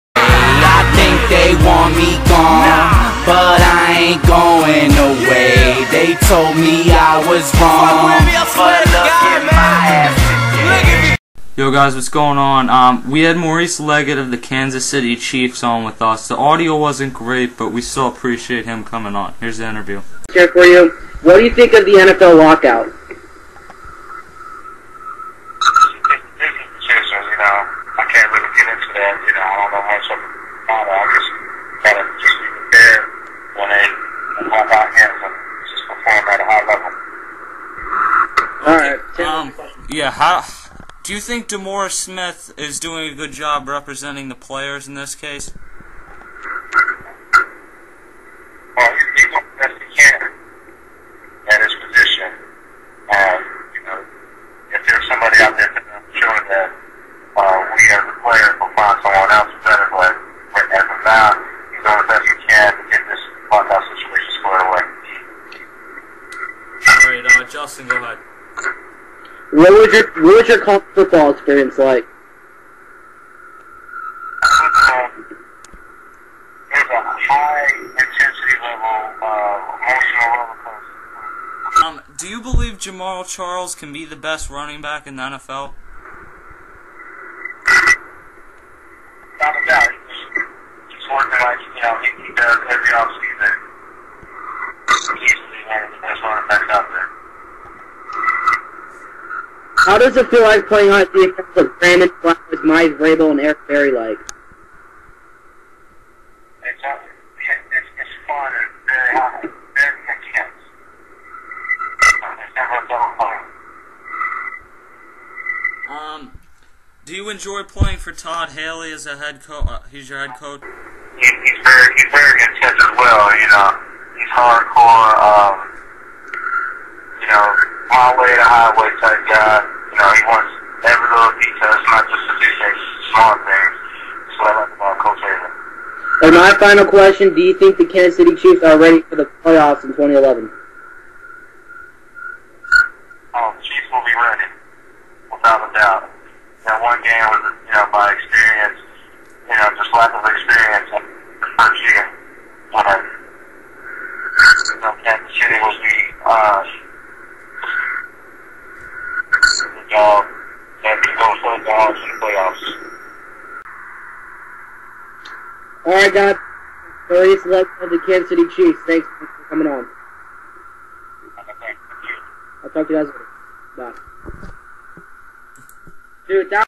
Maurice Leggett (Cheifs CB) "Chiefs will be ready next year"
Chiefs CornerBack Maurice Leggett joined Tim and Justin to talk about the lockout, demaurice smith, Jamaal Charles and much more.

Intro Muic by Novi Novak